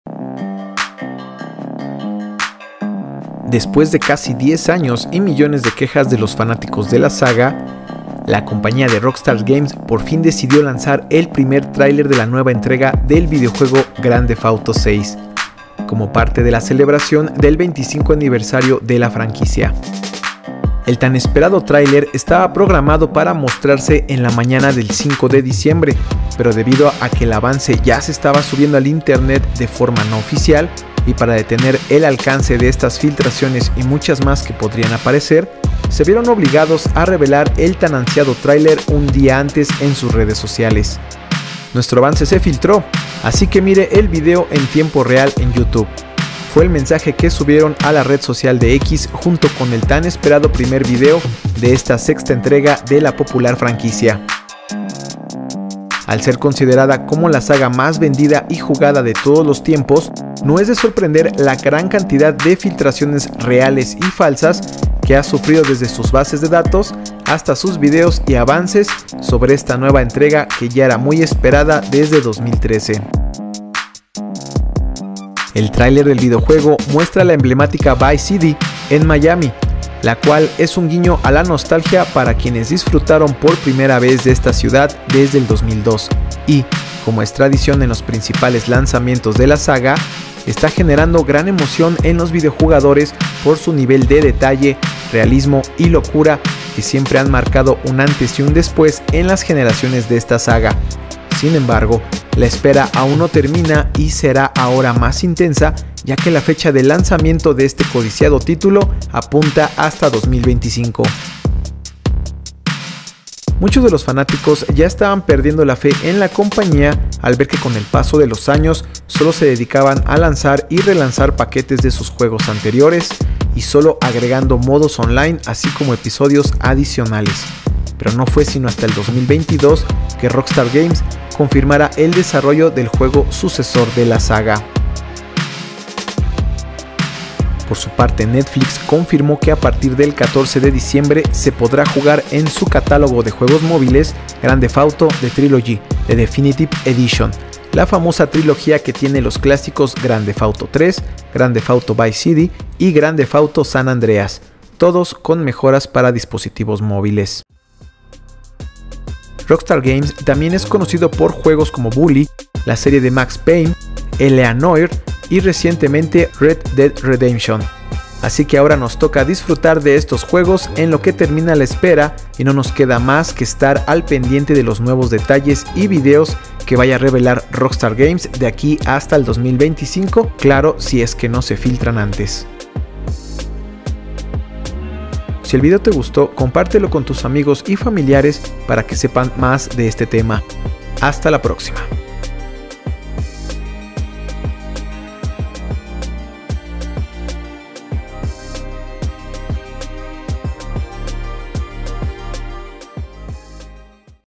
Filtración obliga a subir el trailer oficial de GTA VI
Filtración obliga a subir el trailer oficial de Grand Theft Auto VI

00:00 Diez años de espera
00:21 Razones que obligaron a su liberación
00:41 Mensaje oficial de Rockstar Games
01:15 Contenido del trailer
01:53 Que hacía la compañía durante estos años
02:17 Aviso de Netflix y Rockstar Games
02:39 Otros juegos de la compañía

Después de casi 10 años y millones de quejas de los fanáticos de la saga, la compañía de Rockstar Games por fin decidió lanzar el primer tráiler de la nueva entrega del videojuego Grand Theft Auto VI, cómo parte de la celebración del 25 aniversario de la franquicia.

El tan esperado tráiler estaba programado para mostrarse en la mañana del 5 de diciembre, pero debido a que el avance ya se estaba subiendo al internet de forma no oficial y para detener el alcance de estas filtraciones y muchas más que podrían aparecer, se vieron obligados a revelar el tan ansiado tráiler un día antes en sus redes sociales.

Nuestro avance se filtró, así que mire el video en tiempo real en youtube, fue el mensaje que subieron a la red social de X junto con el tan esperado primer video de esta sexta entrega de la popular franquicia

Al ser considerada como la saga más vendida y jugada de todos los tiempos, no es de sorprender la gran cantidad de filtraciones reales y falsas que han sufrido desde sus bases de datos hasta sus vídeos y avances sobre esta nueva entrega que ya era muy esperada desde 2013

El tráiler del videojuego muestra la emblemática Vice City, en Miami, la cual es un guiño a la nostalgia para quienes disfrutaron por primera vez de esta ciudad desde 2002 y, como es tradición de los principales lanzamientos de la saga, está generando gran emoción en los videojugadores por su nivel de detalles, realismo y locura que siempre han marcado un antes y después en las generaciones de esta saga. Sin embargo la espera aún no termina y será ahora más intensa ya que la fecha del lanzamiento de este codiciado título apunta hasta 2025.

Muchos de los fanáticos ya estaban perdiendo la fe en la compañía al ver que con el paso de los años solo se dedicaban a lanzar y relanzar paquetes de sus juegos anteriores y solo agregando modos online así como episodios adicionales, pero no fue sino hasta 2022 que rockstar games confirma el desarrollo del juego sucesor de la saga.

Por su parte Netflix confirmó que a partir del 14 de diciembre se podrá jugar en su catálogo de juegos móviles Grand Theft Auto: The Trilogy - The Definitive Edition, la famosa trilogía que tiene los clásicos Grand Theft Auto III, Grand Theft Auto: Vice City y Grand Theft Auto: San Andreas, todos con mejoras para dispositivos móviles.

Rockstar Games también es conocida por juegos como Bully, la serie de Max Payne, L.A. Noire y recientemente Red Dead Redemption, así que ahora nos toca disfrutar de estos juegos en lo que termina la espera y no nos queda más que estar al pendiente de los nuevos detalles y videos que vaya a revelar Rockstar Games de aquí hasta 2025, claro si es que no se filtran antes.